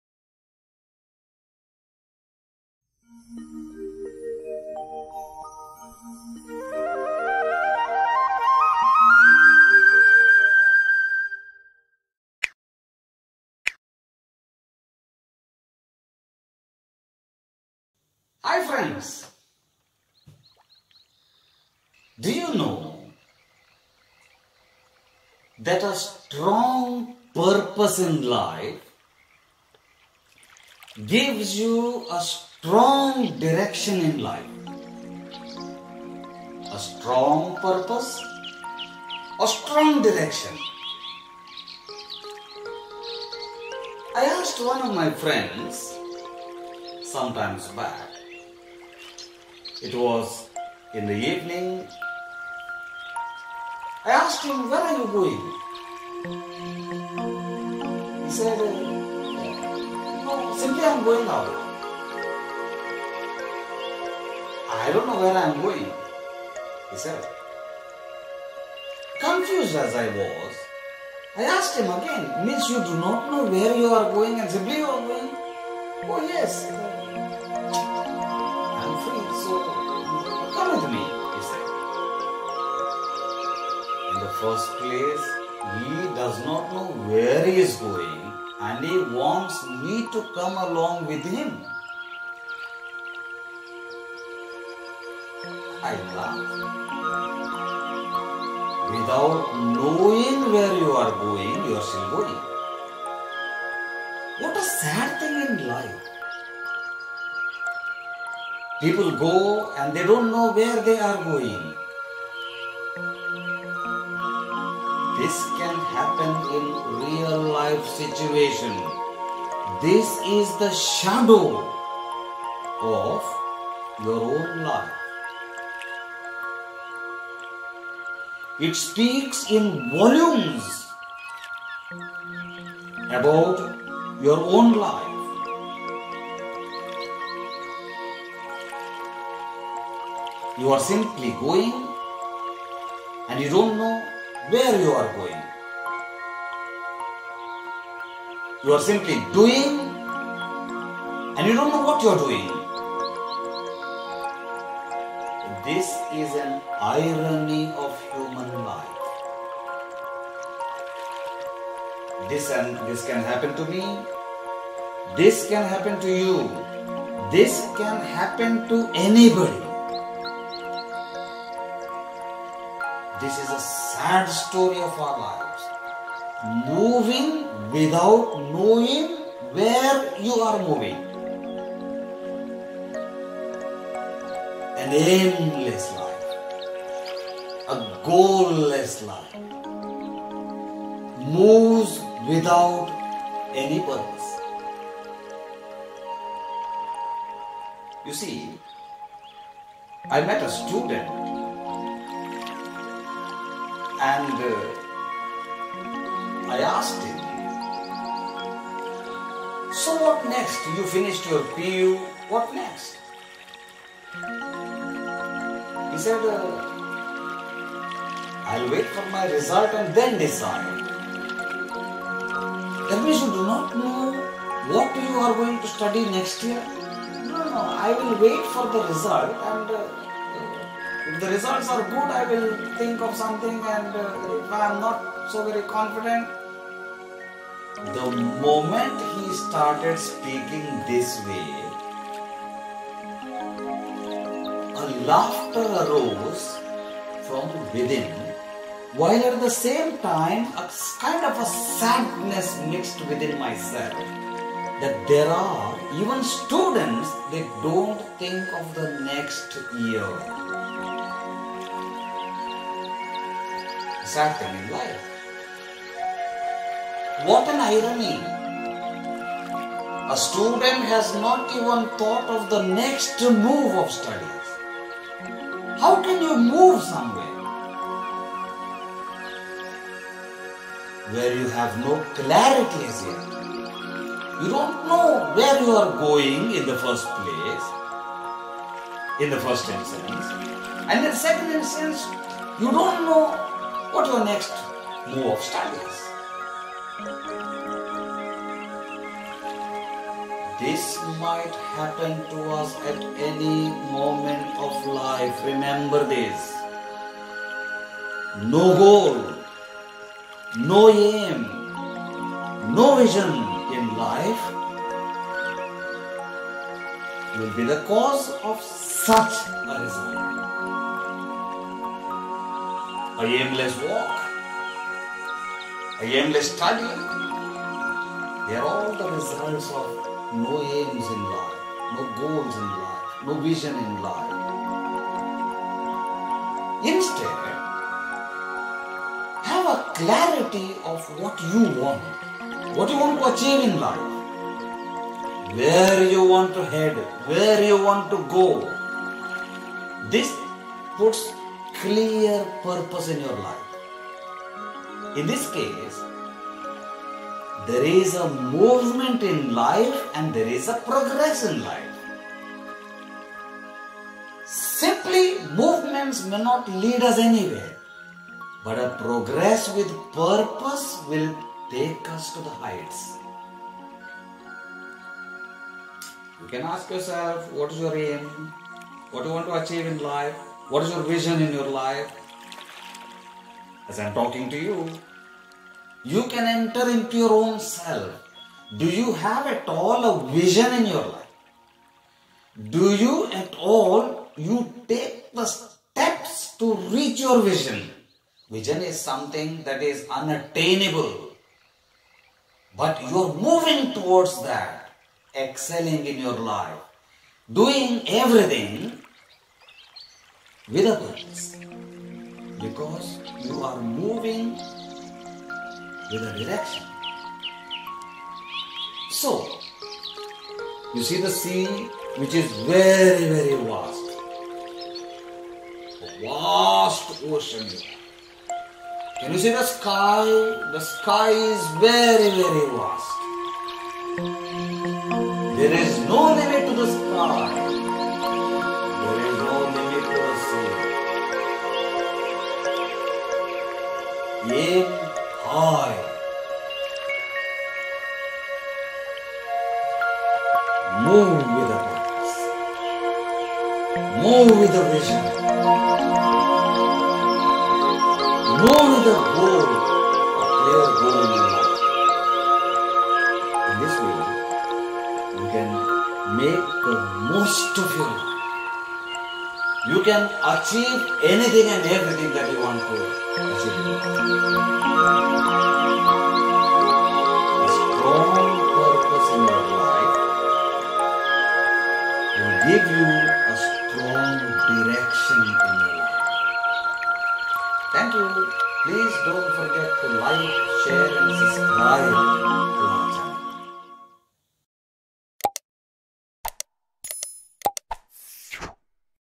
"Have a purpose and lead a focused life"
Purposeful life may emerge as a state of bliss in a person’s life. All depends on whether one has a purpose in life or not. Aimless life, is a drifting life. As the wind blows to any direction, so it might happen to a person without an aim and a goal. With examples, it is my desire to enlighten the public that life is worth living, and hence live a life with a purpose.